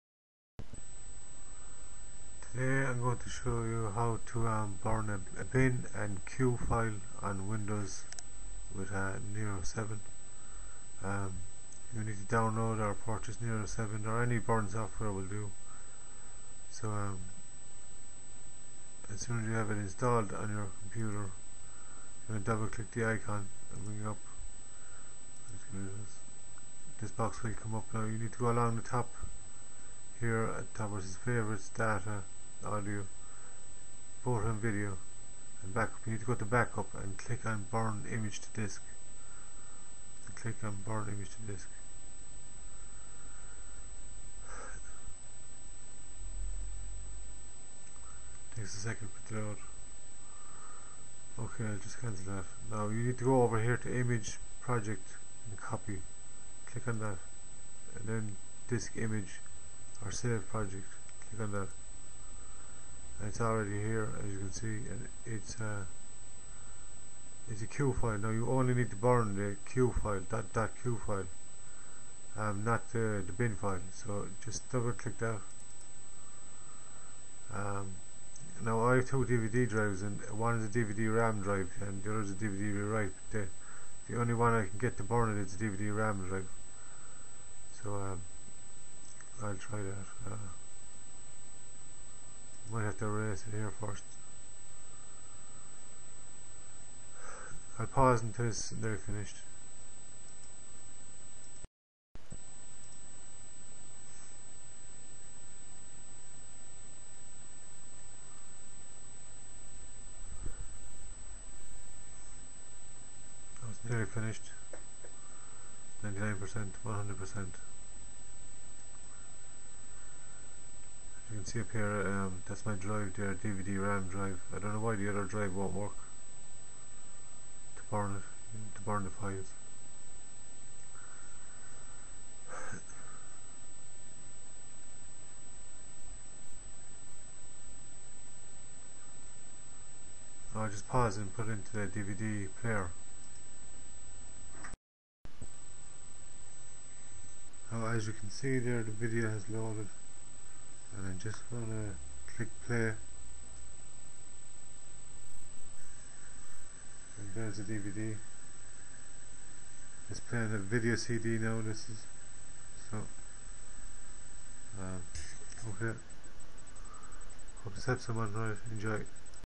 How to burn .bin and .cue files on windows with NERO7
How to burn .bin and .cue files on windows7 with NERO7. You need only load the .cue file into your software as shown in video and then burn the .cue file it will automatically burn the .bin file so dont select the bin file select the .cue file when you want to burn it..this type of file is usually a VCD(video cd) file.